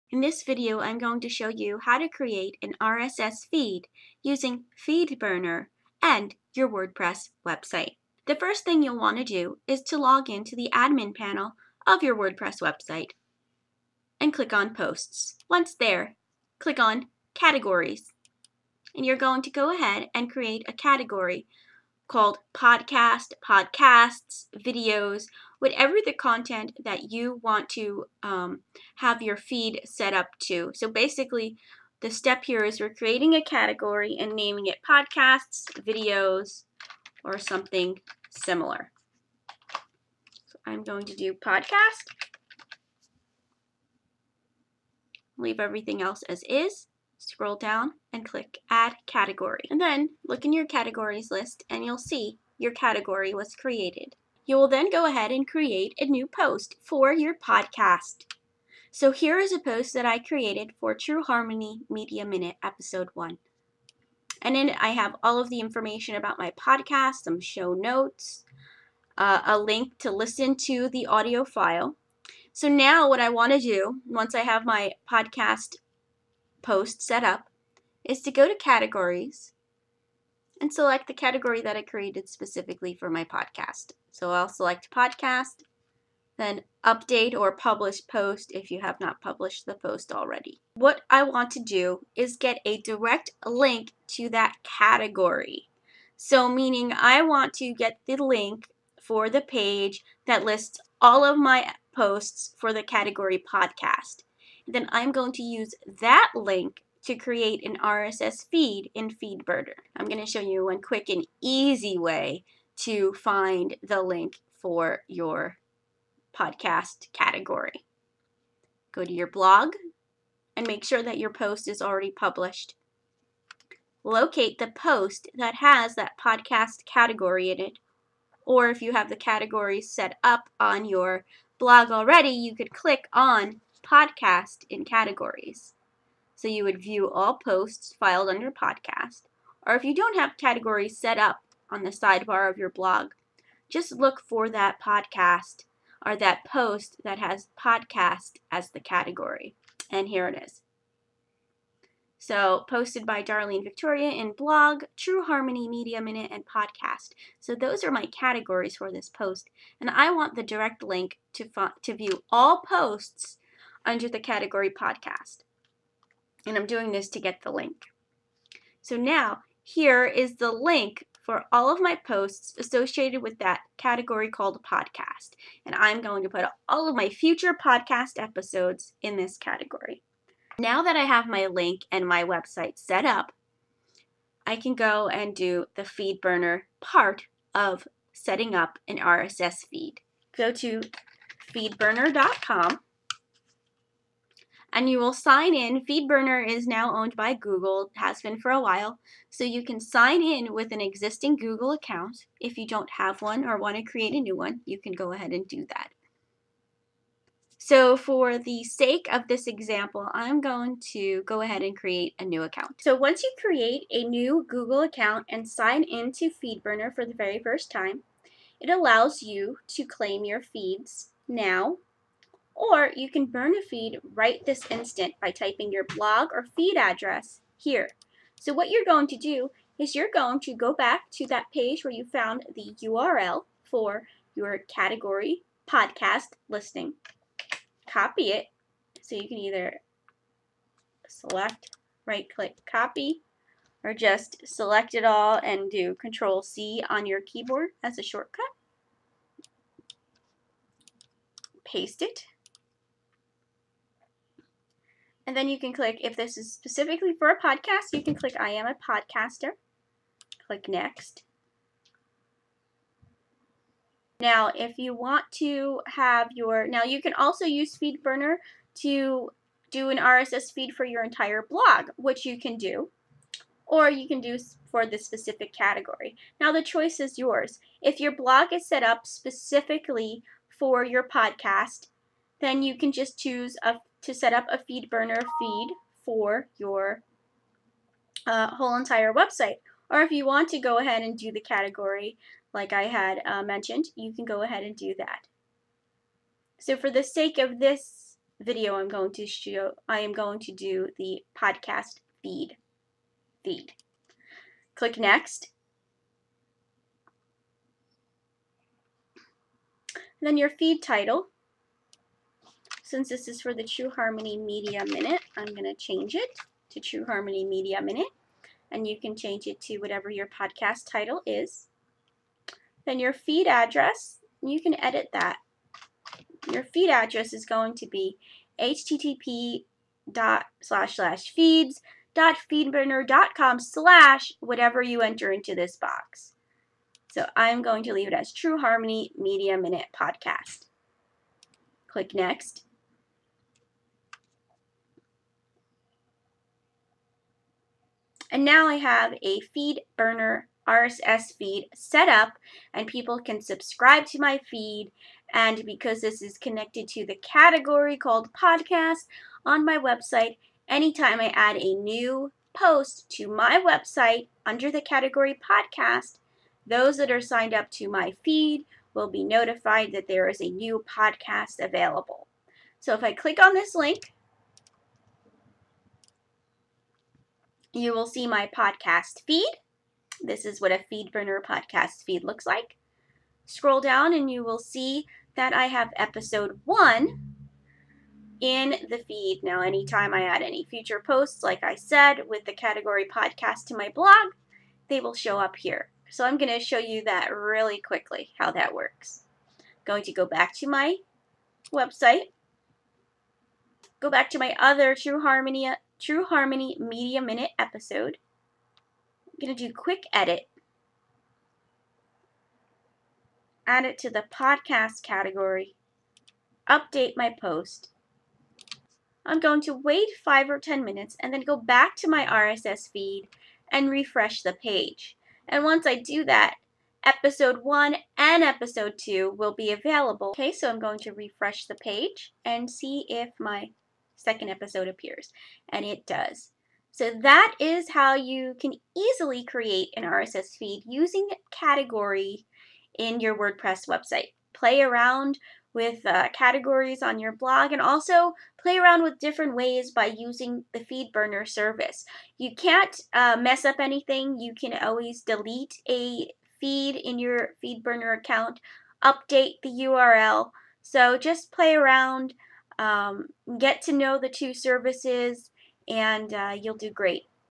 How to create a Podcast RSS Feed using Wordpress and Feedburner
This video shows you how to utilize categories on your Wordpress website and then use the category listing page url to create a RSS  Feed using Feedburner.